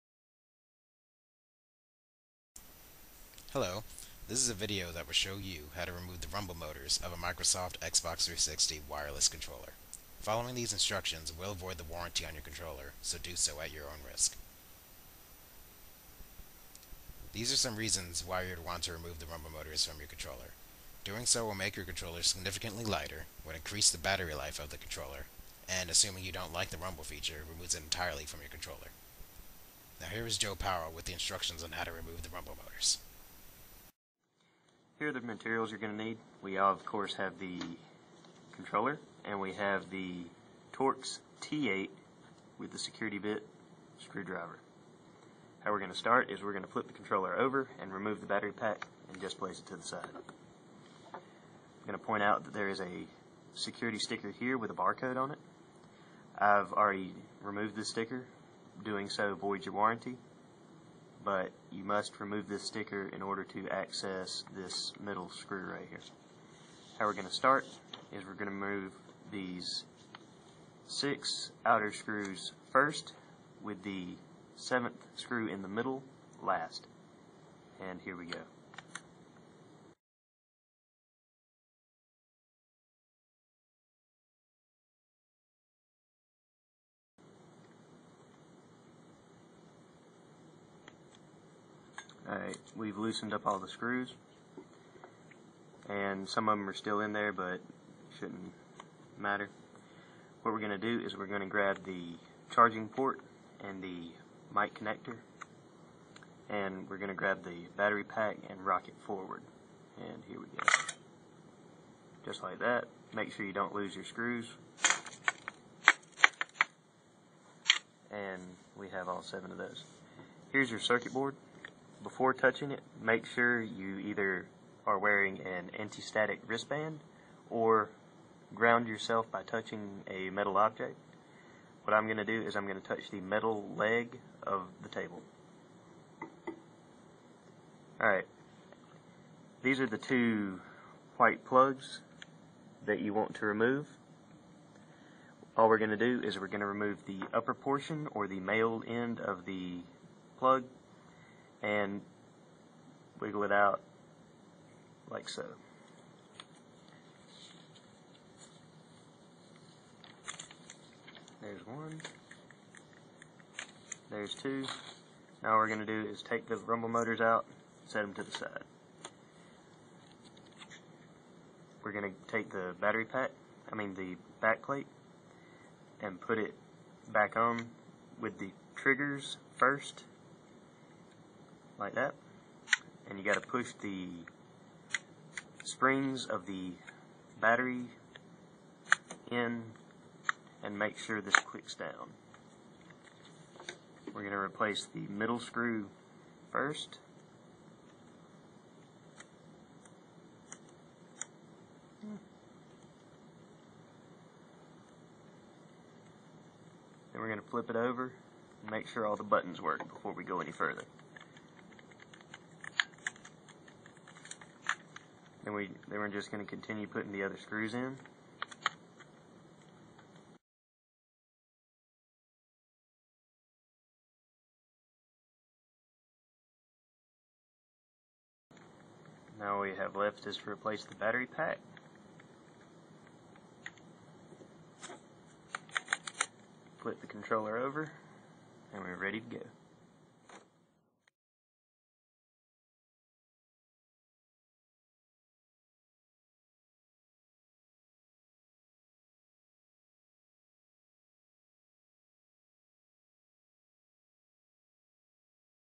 Removing the Rumble Motors of a Microsoft Xbox 360 Wireless Controller
This video contains step by step instructions on how to remove the rumble motors of your Xbox 360 controller.